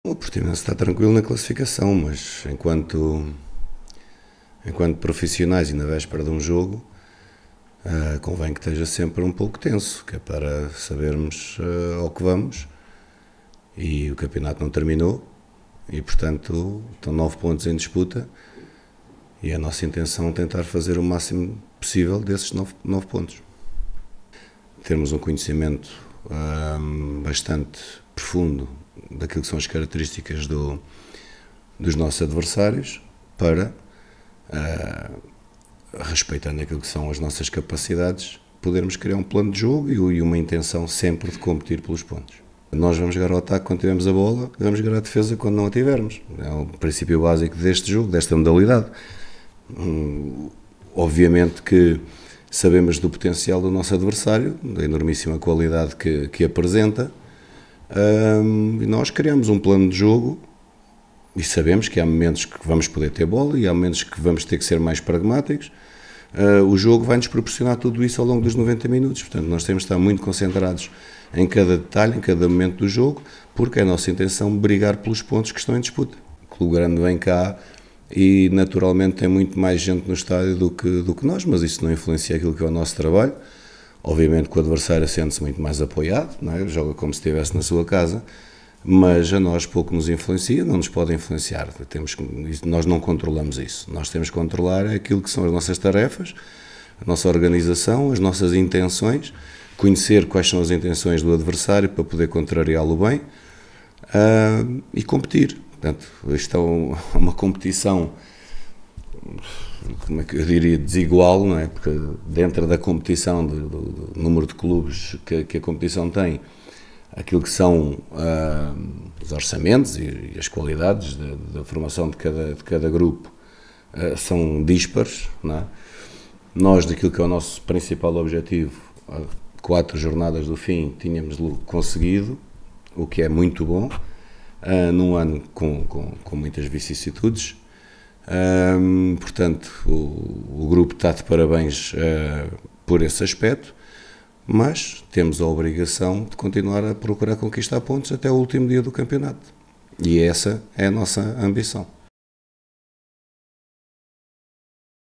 Paulo Sérgio quer Portimonense concentrado em cada detalhe e espera o “melhor” Benfica
O treinador do Portimonense pediu hoje aos seus jogadores concentração em cada detalhe do jogo de sábado frente ao Benfica, notando que a luta pelo título da I Liga portuguesa de futebol não diz respeito aos algarvios.

Edição de João Pedro Correia